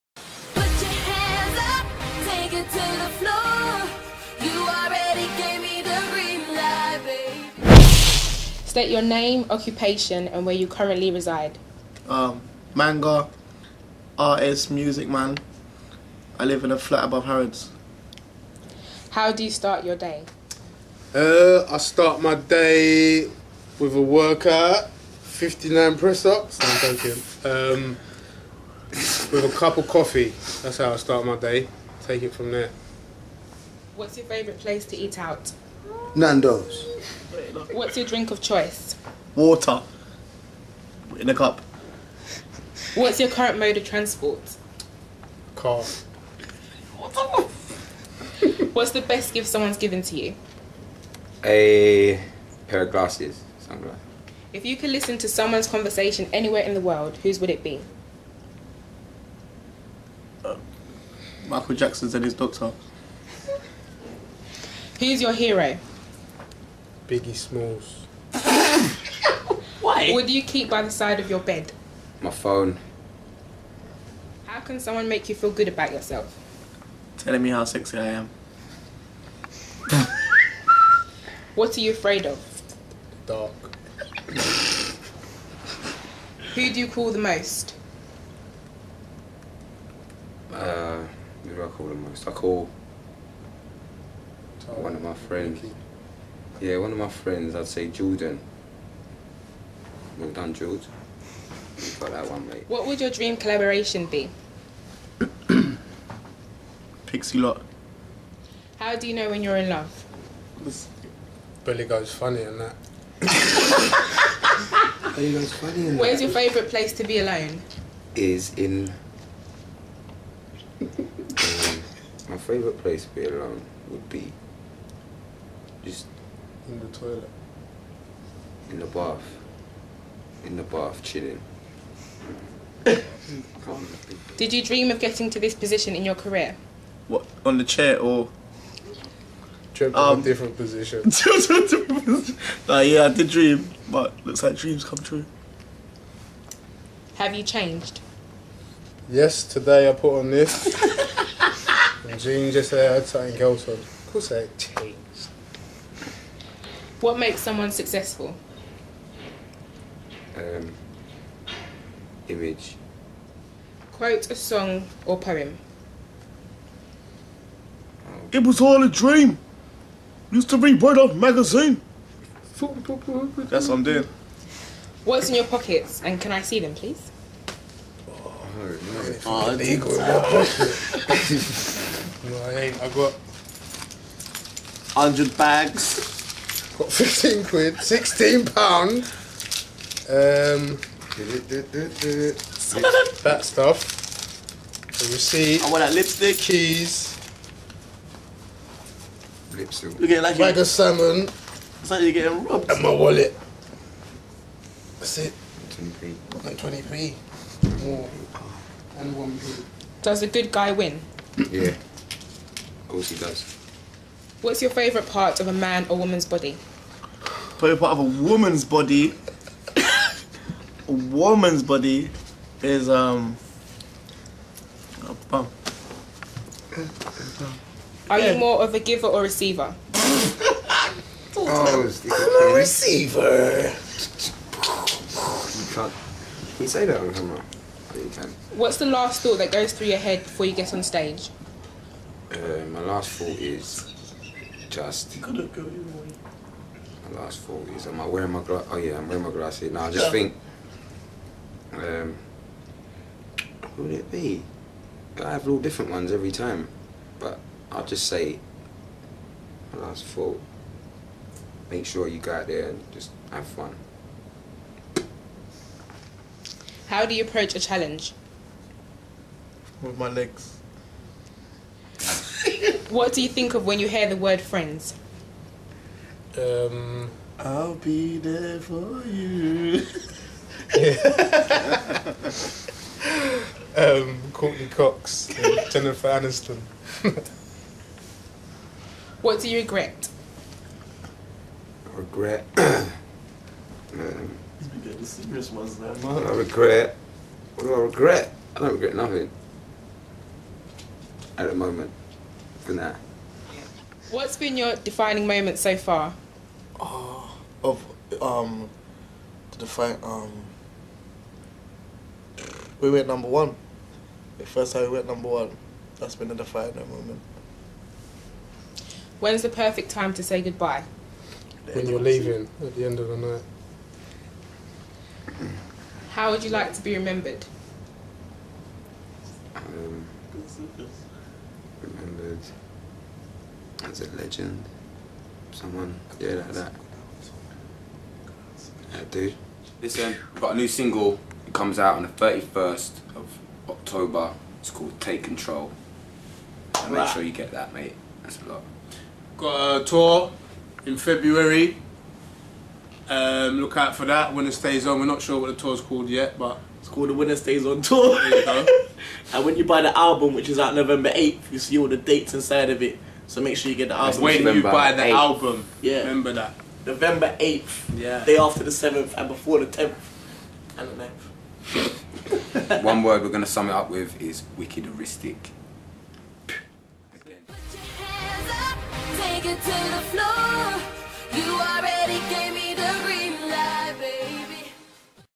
Roll Deep - 'On The Spot' - Interview
DropoutUK.com catch up with the UK's favorite grime crew, Roll Deep! The boys give us their 'Dropout 30' Interview!

We have a right laugh with Manga, Breeze and Scratchy as they tell us how they start their day, their favorite place to eat and how you know when your in love.

Directed, Edited & Filmed by Danny Lowman [twitter @dannylowman]

Presented By: Maria Okanrende.